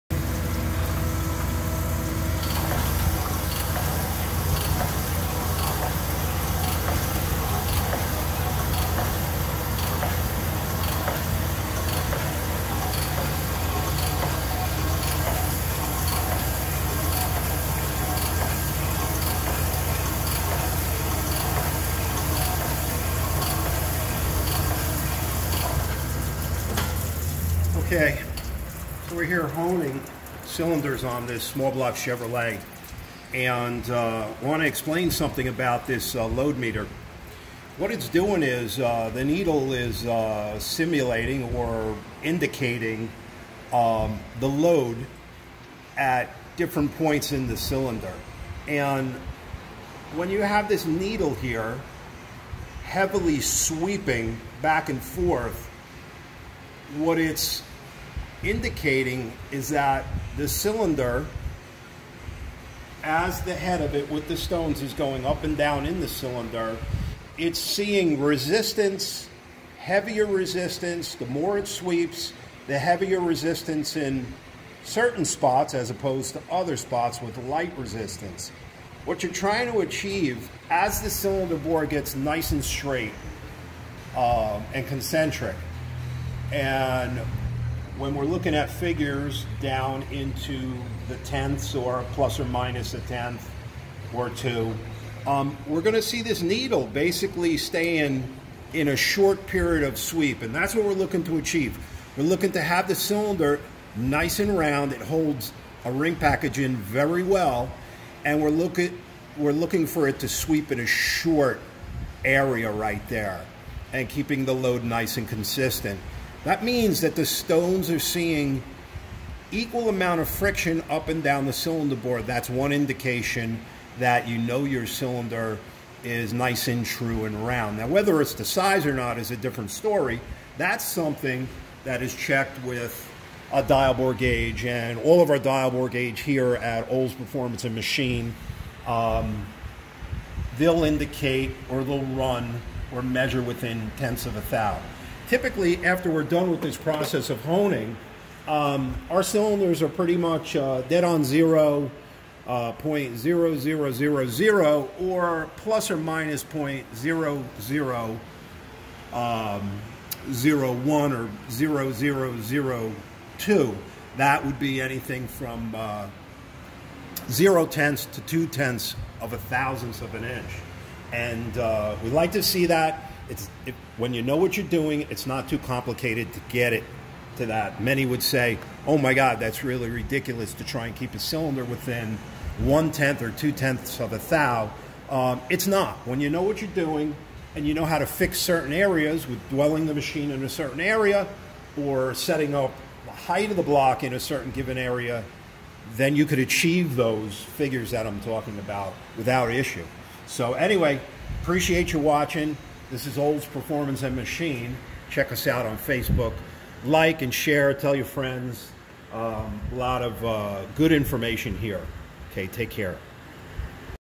Sunnen CV 616, CK 10 Honing
Explanation of how the load meter works on a Sunnen CK 10 or CV 616.